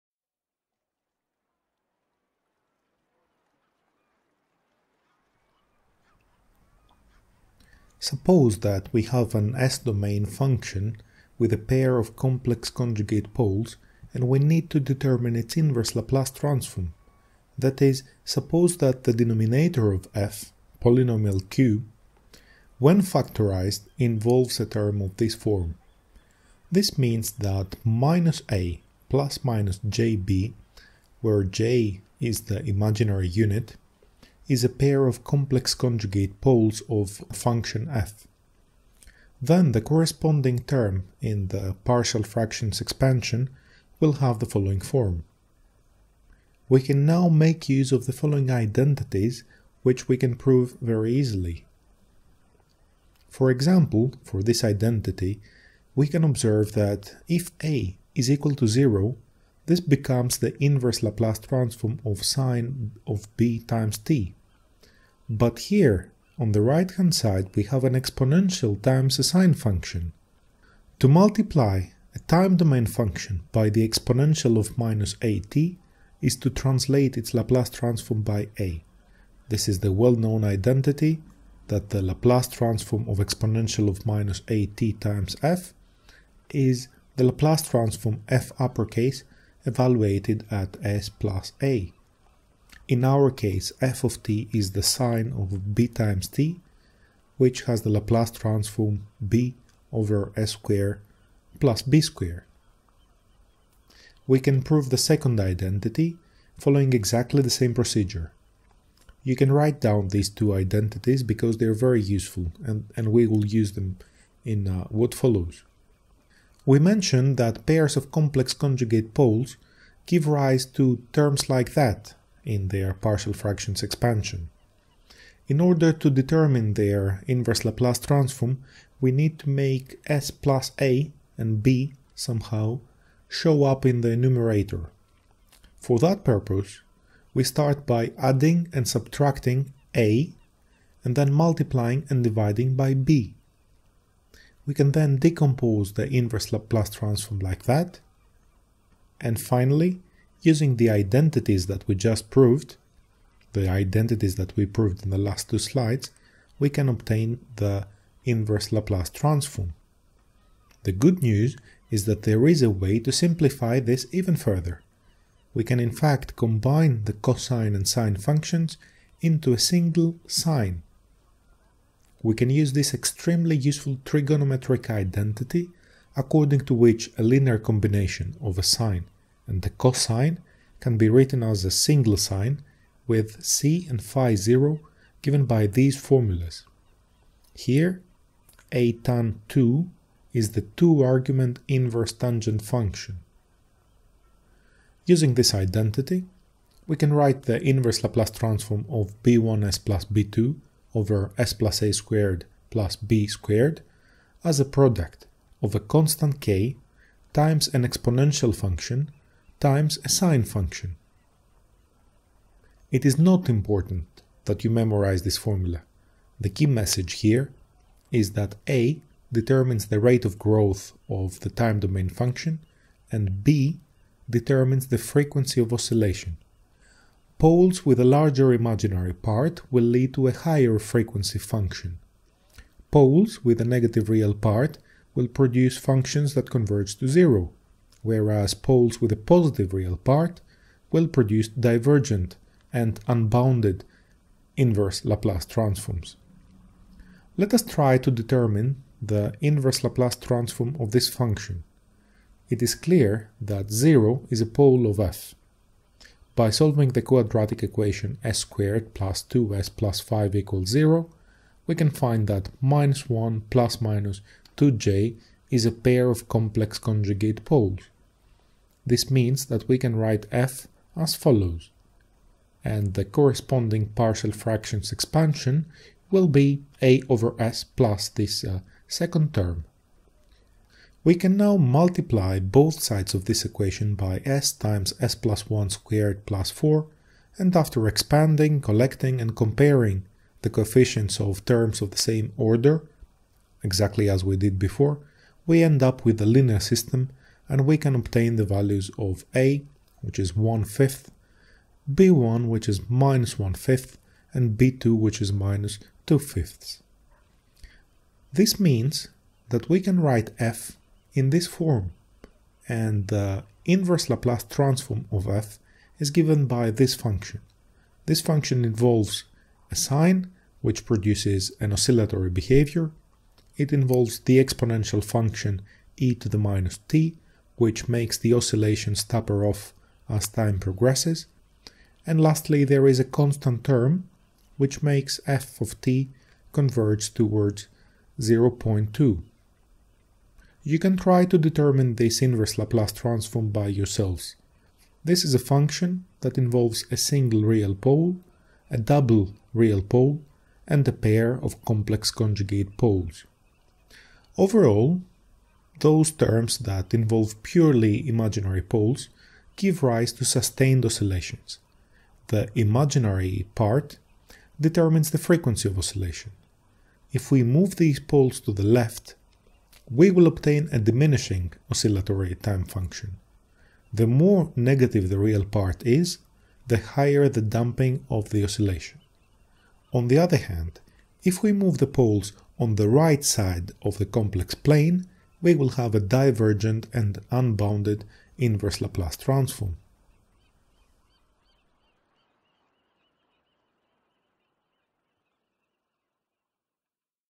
Inverse Laplace Transform - Complex Conjugate Poles
We determine the inverse Laplace transform of strictly proper rational functions with complex conjugate poles. Once again, we use an appropriate partial fractions expansion.

▬ Control Systems Textbook  ▬▬▬

Other countries:
The Netherlands